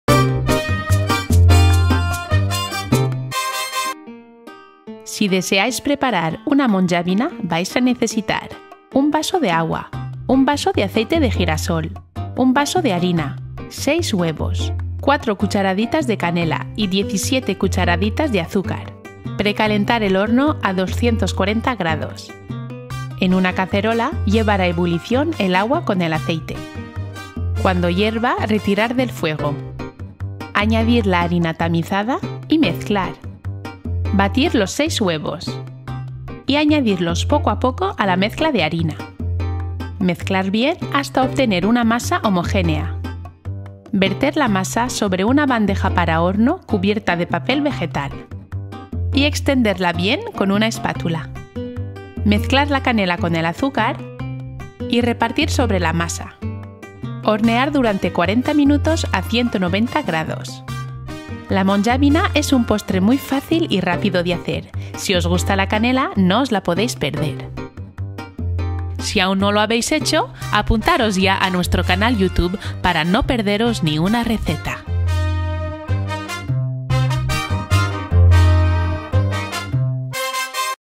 Receta de la Monjavina
Para preparar una Monjavina solo necesitaréis agua, aceite de girasol, harina, huevos, canela y azúcar. Con productos modestos obtendréis un postre espectacular. Si os gusta la canela y los dulces esta es la receta ideal. La Monjavina es de origen árabe y se hace en Ontinyent y en la zona de la Vall d'Albaida (Valencia). Un postre encantador que es además muy fácil y rápido de hacer. Al hornearla la masa se hincha y le da una forma muy peculiar. Además de estar buenísima la Monjavina es una receta original que sorprenderá a vuestros invitados. En un momento, tendréis un postre delicioso, esponjoso y sabroso. Venga, preparadlo ya !

Ingredientes para la Monjavina :

- 1 vaso de agua
- 1 vaso de aceite de girasol
- 1 vaso de harina
- 6 huevos
- 4 cucharaditas de canela
- 17 cucharaditas de azúcar